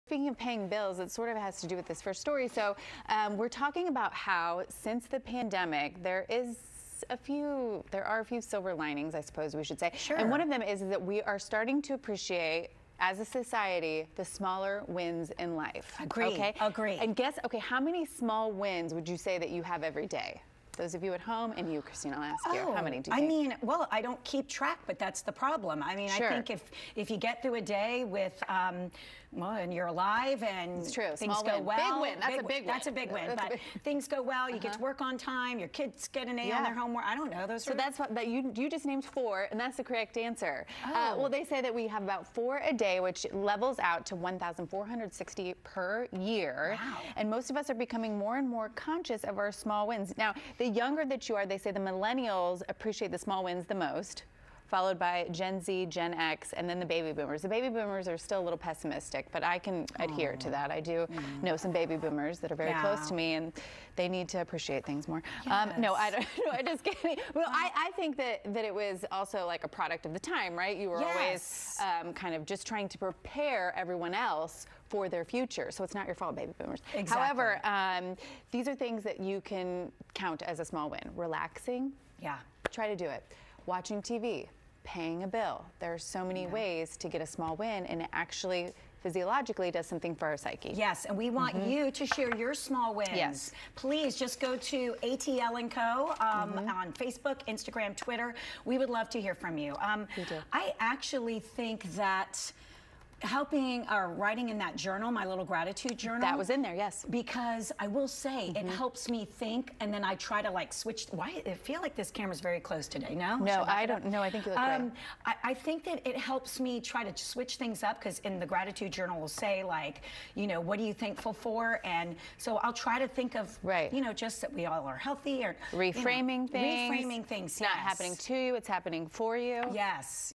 Celebrating "Small Wins"
Do you celebrate the little victories in your everyday life? Christine and Cara share why more are taking note of the small wins.